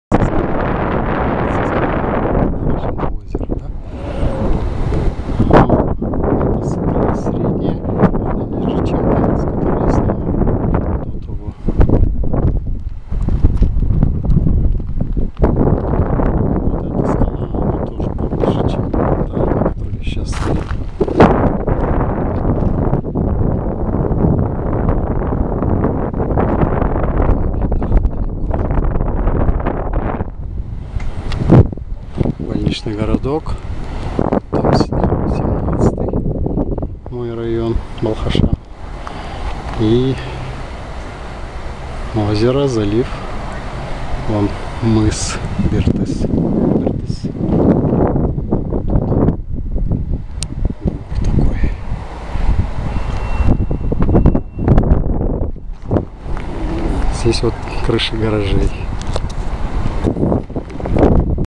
На середине горы в центре города Балхаш - виды с этой точки
На этом видеоролике находимся на середине горы в центре города Балхаш. С этого ракурса виды несколько другие. Внизу видны двухэтажные дома на улице Сейфуллина. Эта гора возвышается прямо среди домов в городе Балхаше. Она находится в квартале, который очерчен четырьмя улицами. Это улицы Мира, Сейфуллина, Амангельды и Желтоксан. Ближе всего эта гора находится к улице Сейфуллина. Это место раньше к центру не относилось, но город Балхаш всё таки растёт и расширяется. Поэтому я решил всё же назвать это центром города. Во всяком случае центр города очень близок. Можно с этой точки обозревать всё вокруг. Видно далеко.